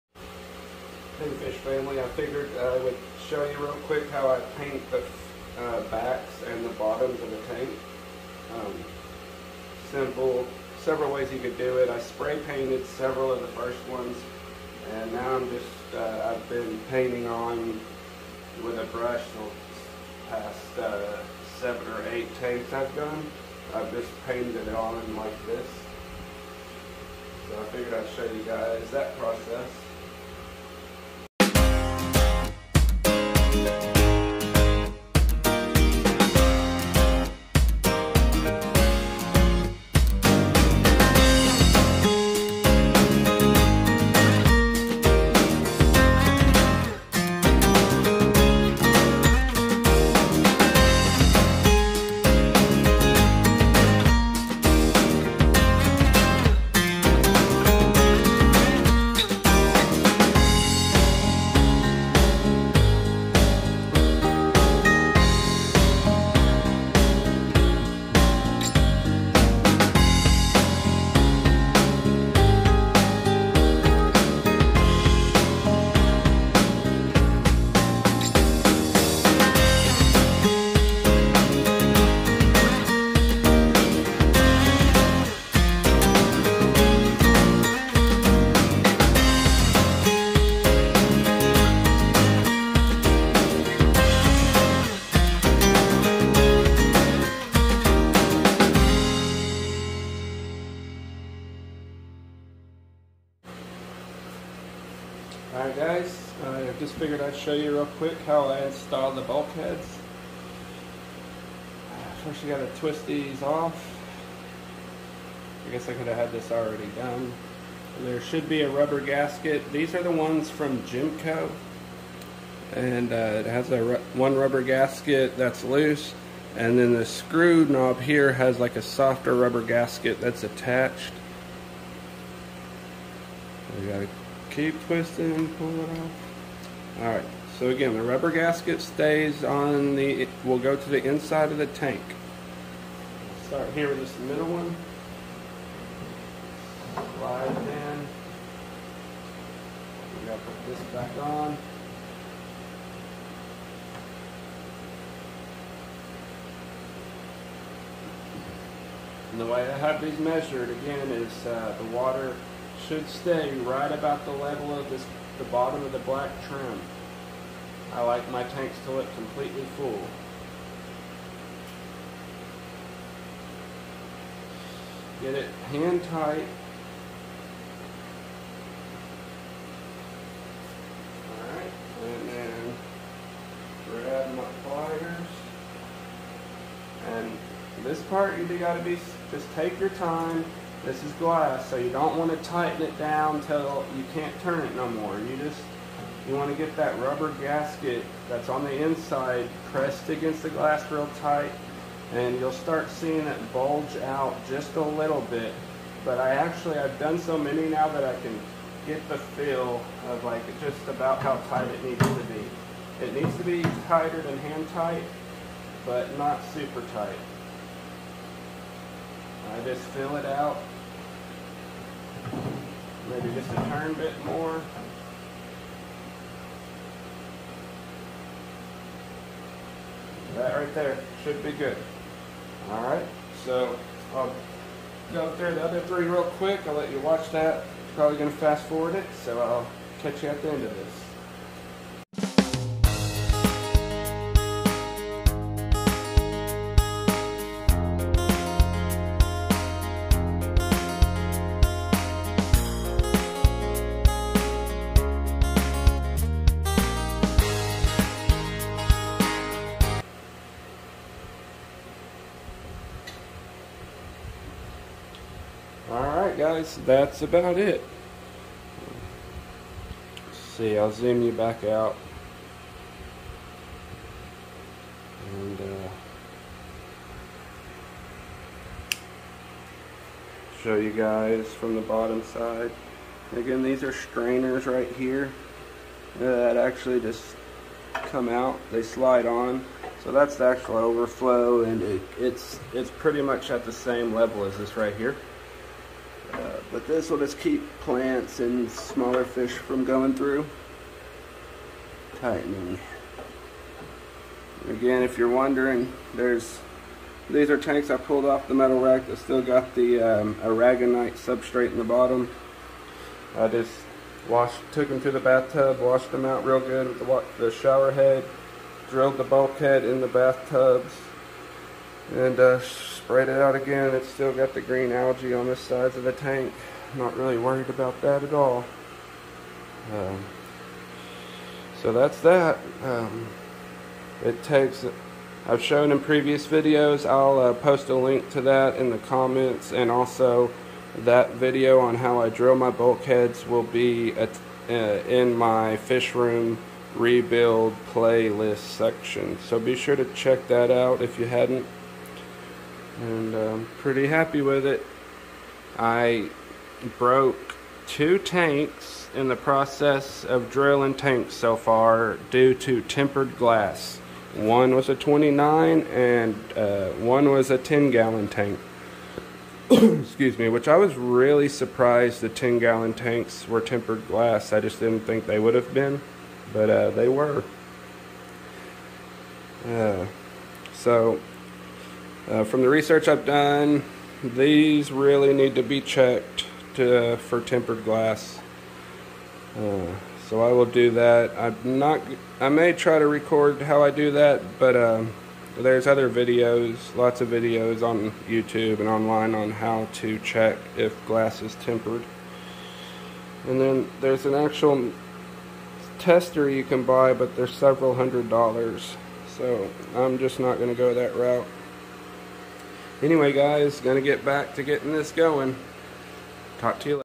Fishroom Rebuild Part 4: Painting The Fish Tanks And Installing Bulkheads
hey fish family thank you for tuning in again on this part for I will be talking about painting the background and underside of the tanks as well as installing the bulkheads for the Overflow system. thank you so much for staying tuned I really appreciate all the support and hope to keep these videos coming!